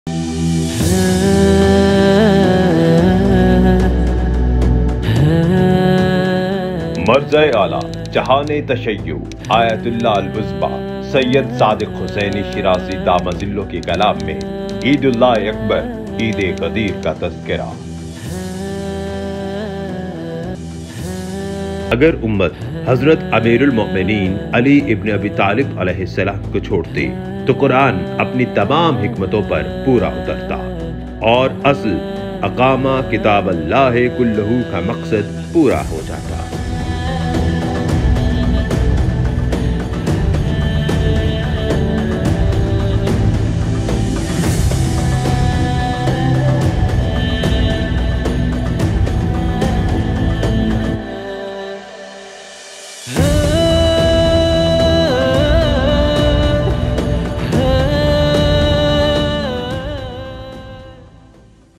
Naqabil e Jubran Nuqsan | Ayatollah Al Uzma Shirazi ka Bayan
🔸مرجع اعلیٰ جہانِ تشیع آیت اللہ العظمیٰ سید صادق حسینی شیرازی دام ظلہ کے کلام میں عيدالله اكبر، عيد غدير کا تذکرہ:

عراق

کربلاء

کربلاء مقدسہ

زیارت

زیارت گاہ

آفیشل ویبسائٹ

ایمیل

ایڈریس